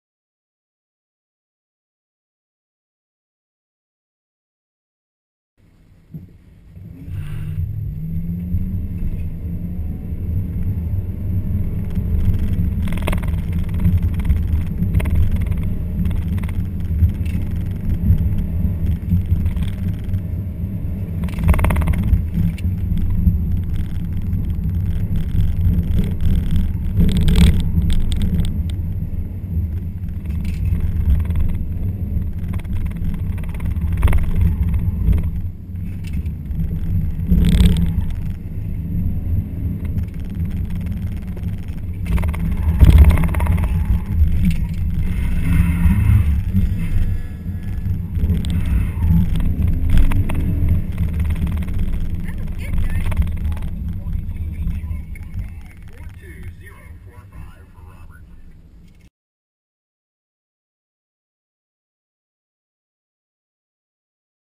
09.20.2015 SIRSCCA Event.
One run of the SIR SCCA Autocross event at Lawrence County Mid-America Airport.